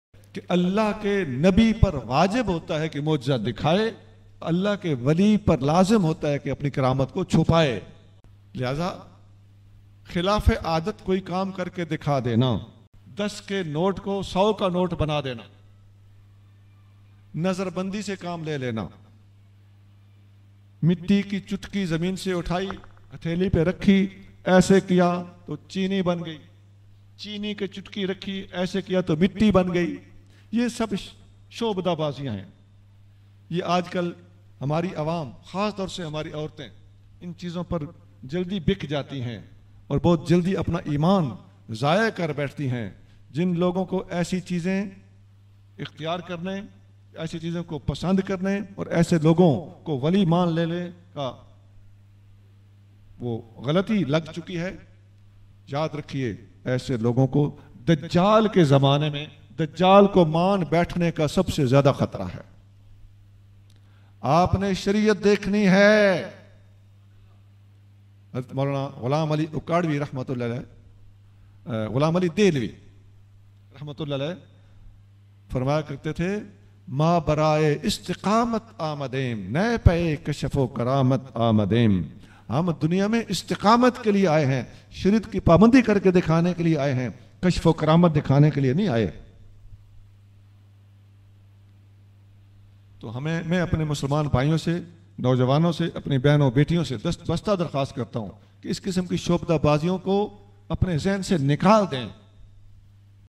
Mojza Dikhana Wajib Hai Aur Karamat ko Chupana Lazim Hai Peer Sain Ghulam Rasool Qasmi Nashbandi Q
📡📡📡 Watch Live Mehfil
Ghaazi Islamic Production
For All Kinds of Islamic Event Coverage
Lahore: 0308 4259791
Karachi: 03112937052